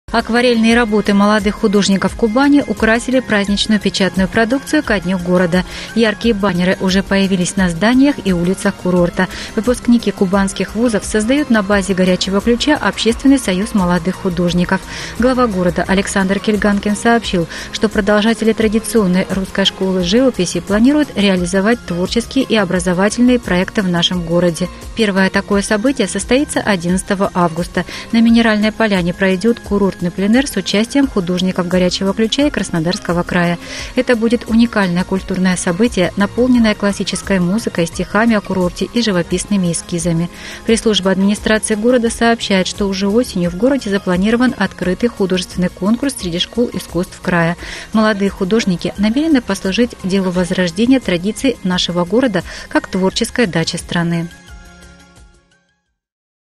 Акварельный Горячий Ключ
Акварельный Горячий Ключ украсит юбилейный День города и появится на праздничной печатной продукции. Эскизы стали творческим подарком курорту от молодых художников Кубани.